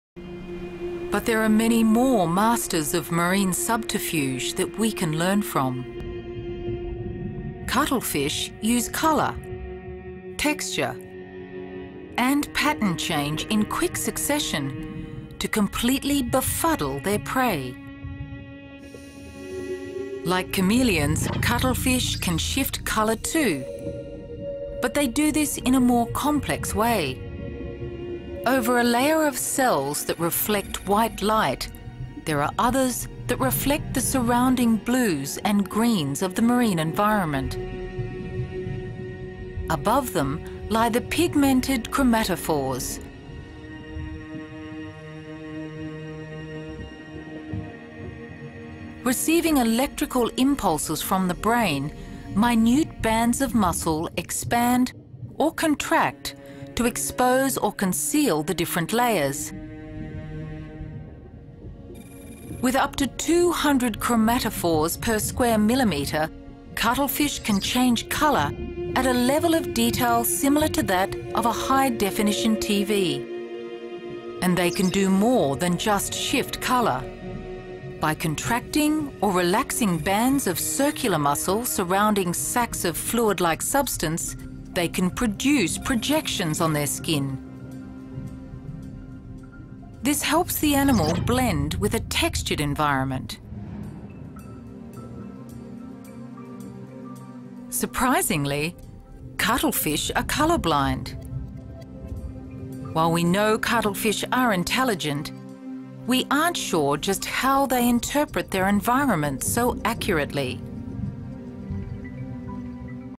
Cuttlefish
Cuttlefish are marine animals of the order Sepiida. They belong to the class Cephalopoda, which also includes squid, octopuses, and nautiluses. 'Cuttle' is a reference to their unique internal shell, the cuttlebone